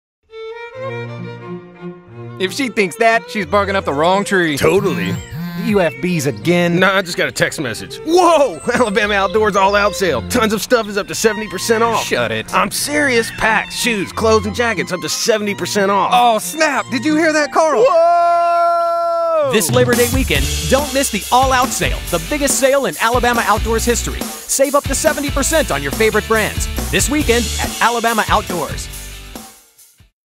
AlabamaOutdoors.mov
Alabama Outdoors TV commercial for the all out sale.